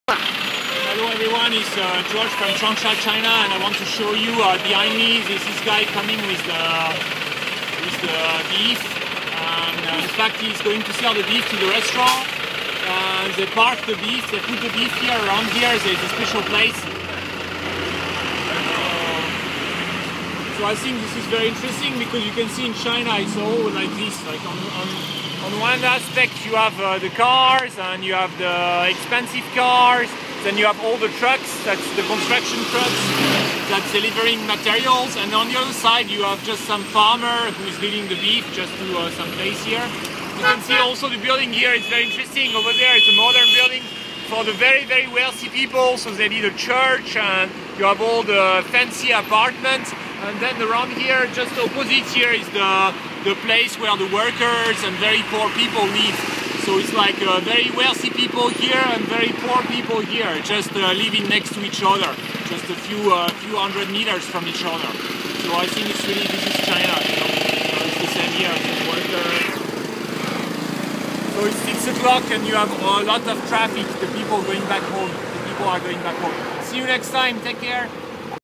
Leading beef to the slaughterhouse in China
China is a gap society. There are many gaps: gaps between wealthy and poor people, gap between modernity and old ways...